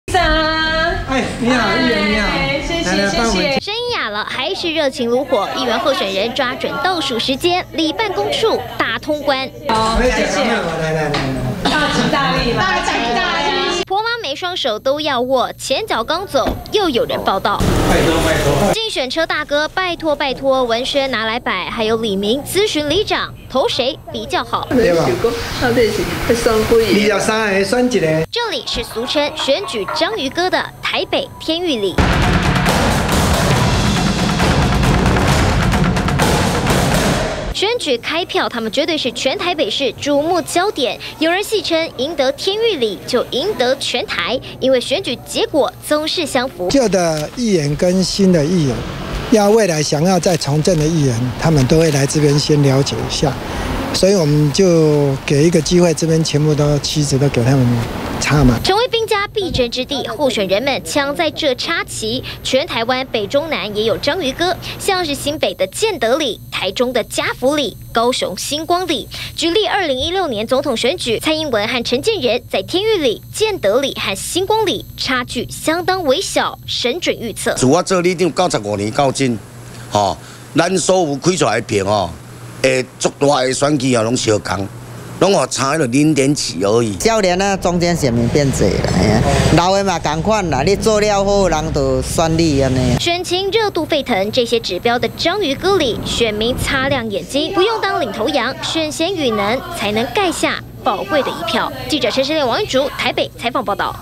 20181120中天新聞 4都選舉「章魚哥」 台北天玉里候選人搶插旗
選戰倒數，全台俗稱「選舉章魚哥」的4個里，也成為矚目焦點，包括台北士林的天玉里、新北市鶯歌建德里、台中家福里和高雄的新光里，被戲稱「得4里贏得天下」，由於每次開票，得票率都激似大選結果，這幾個里，也成為不少候選人，搶攻插旗的地區。